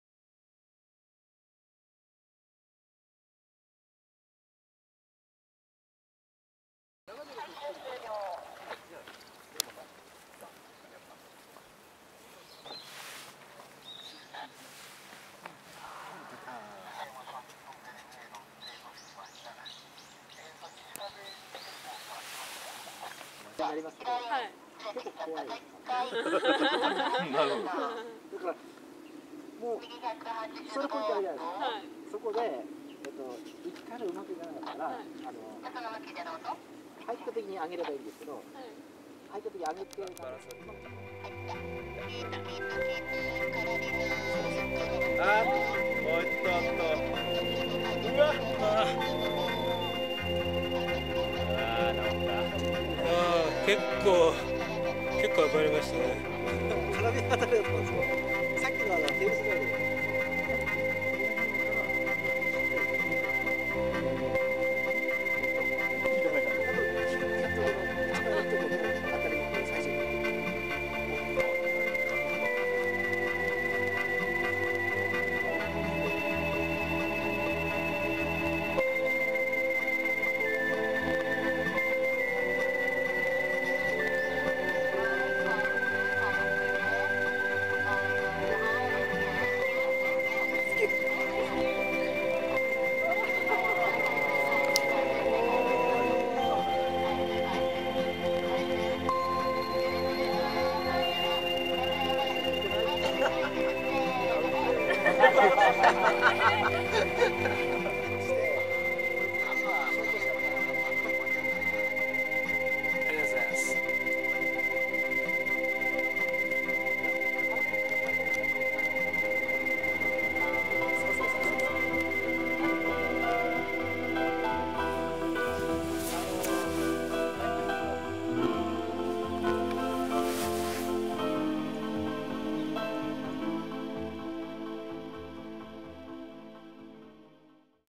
Sky training at Lake Kizaki
praglider accesssky sokai in kizakiko
アクセススカイ蒼海パラグライダースクールの木崎湖セイフティセミナーのひとコマ。